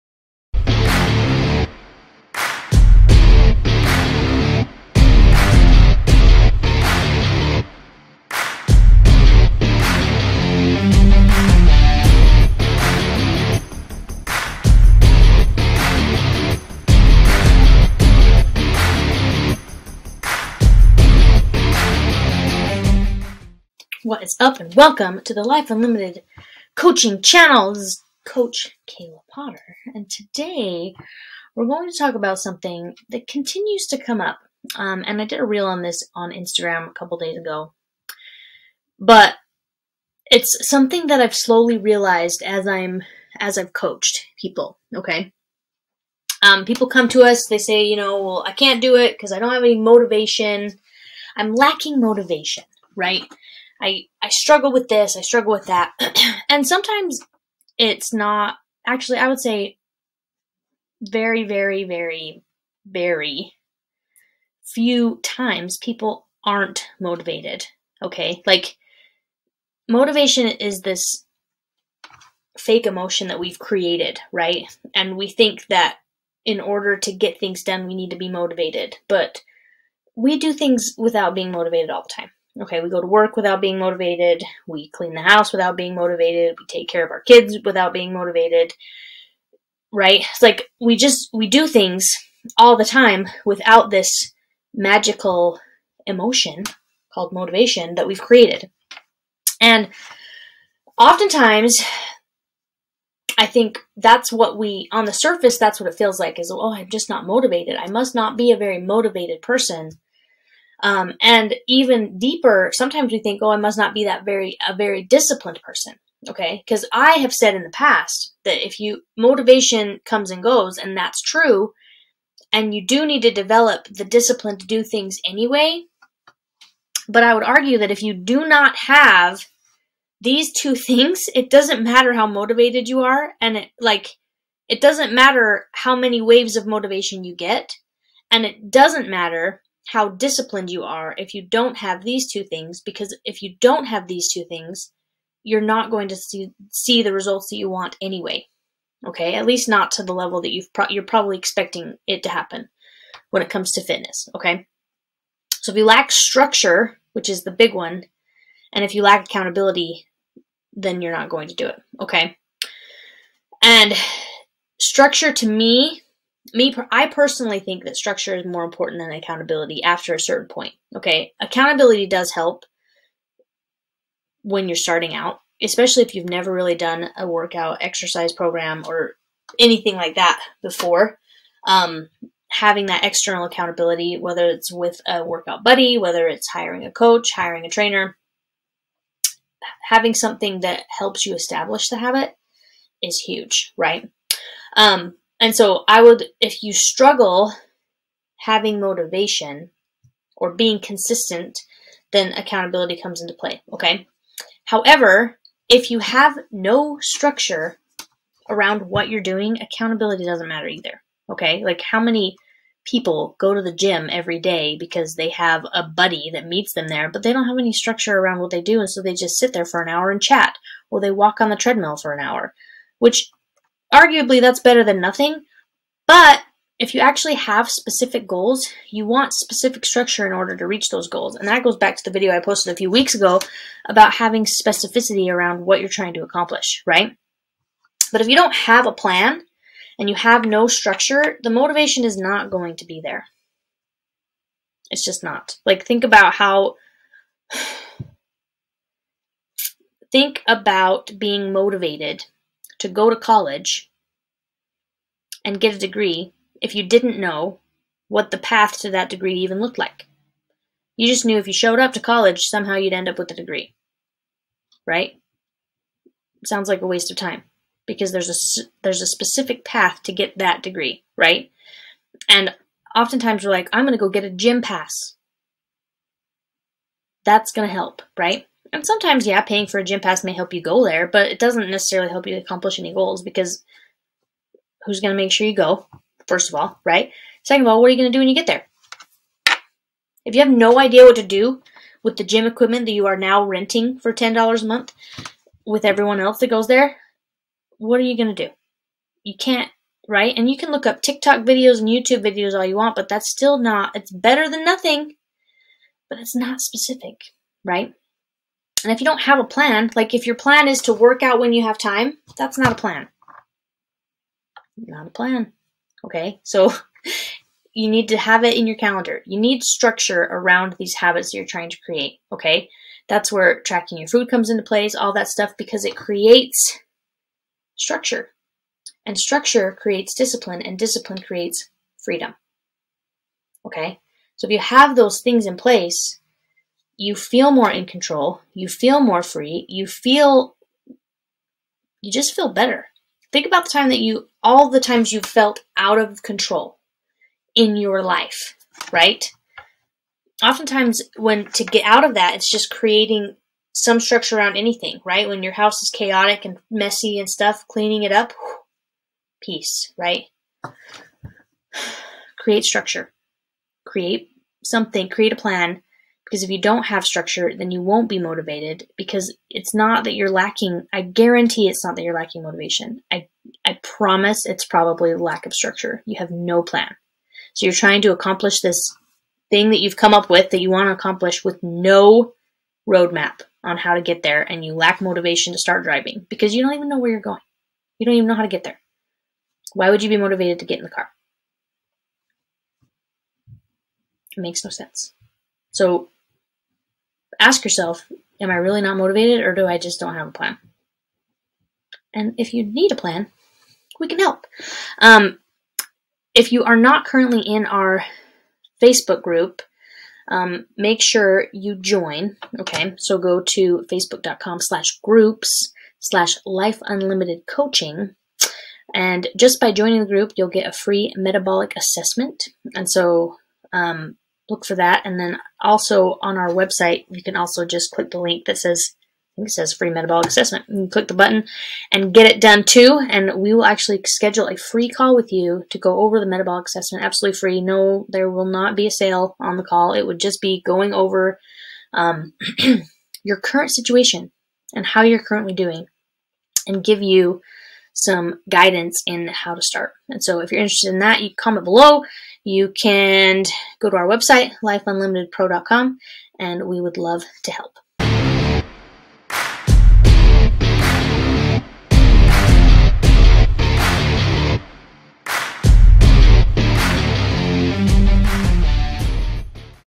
2 Reasons You May Lack Motivation
A lack of motivation may be because you lack two very important factors when it comes to being successful with health and fitness as well as success in other areas of your life. Tune in to find out what these two important factors are.